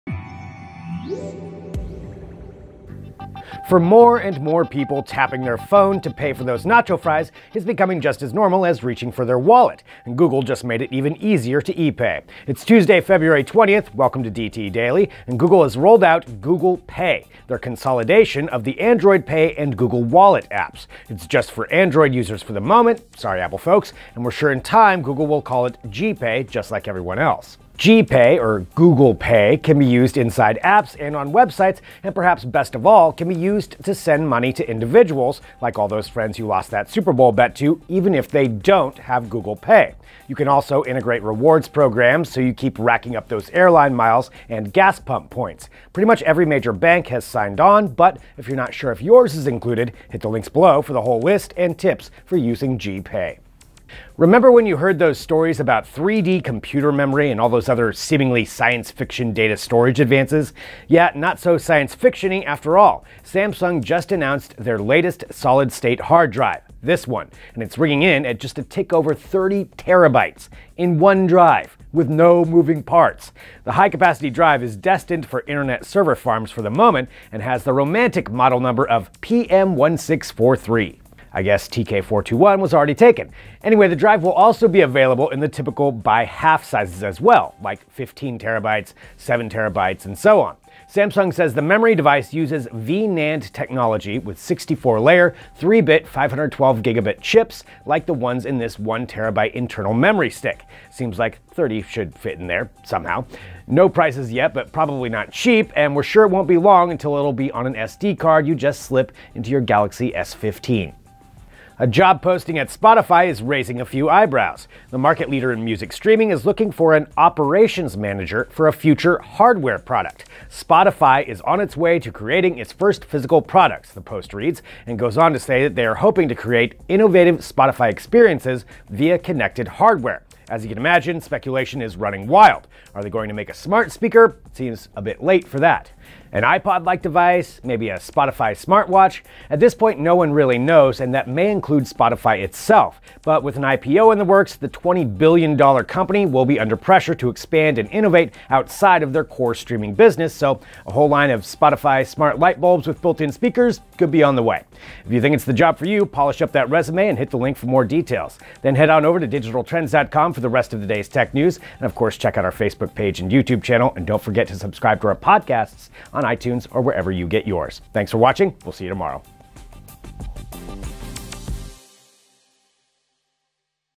Your life on one drive? Samsung debuts 30TB solid state hard drive
Remember when you heard those stories about “3D computer memory” and all those other seemingly science-fiction data storage advances? Yeah, not so science fiction-y after all: Samsung just announced their latest solid state hard drive and it’s ringing in at just a tick over 30 terabytes. In one drive. With no moving parts. The high-capacity drive is destined for internet server farms for the moment and has the romantic model number of PM1643.

The drive will also be available in the typical by-half sizes as well, like 15 terabytes, 7 terabytes and so on. Samsung says the memory device uses V-NAND technology with 64-layer, 3-bit 512-gigabit chips, like the ones in this 1 terabyte internal memory stick. No prices yet but probably not cheap, and we’re sure it won’t be long until it’ll be an SD card you just slip into your Galaxy S15.